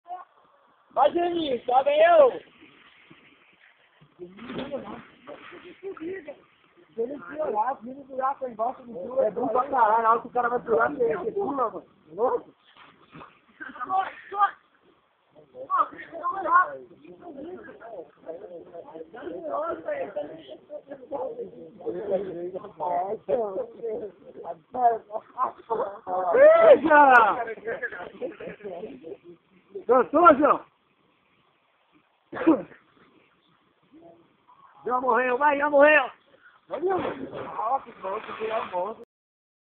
cachoeira salesopolis 01-02-2009 xingu
cachoeira da velha em salesopolis
xingu pulando de cima de uma arvore